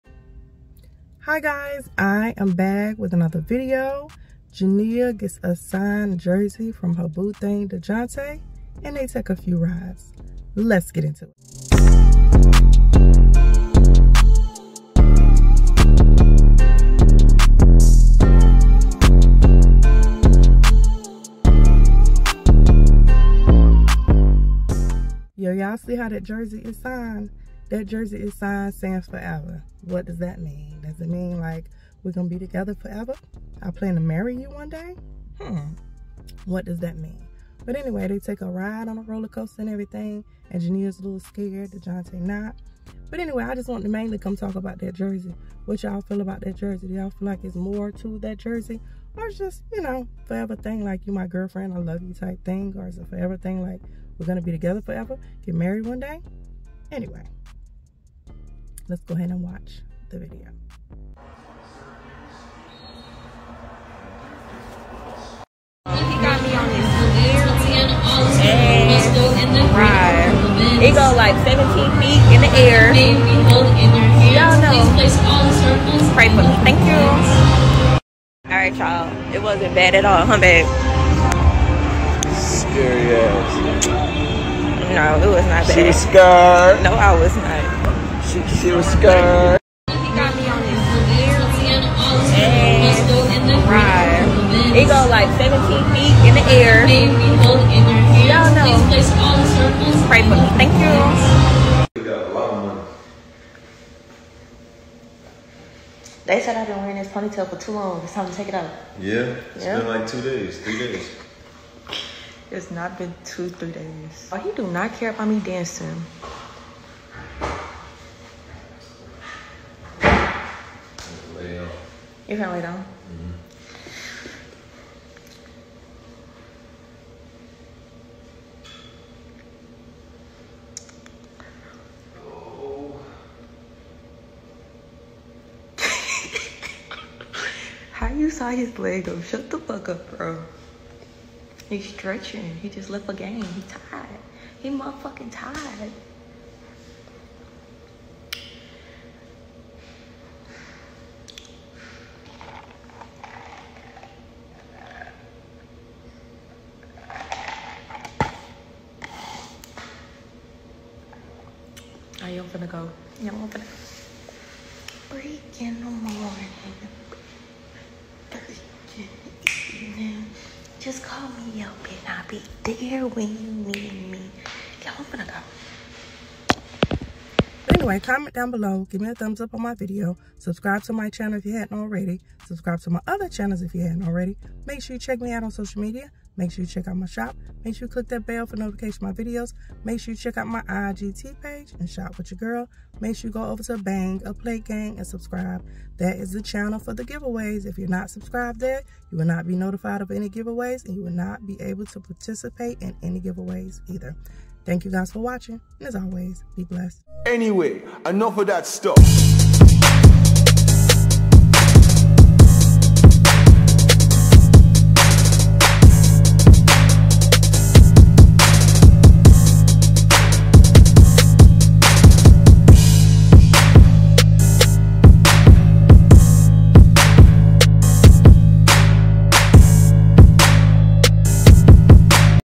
Jania gets a Signed Jersey from Dejounte that says We Locked in 4 Life. Marriage??
💞 Follow Me 💞

💞 Support 💞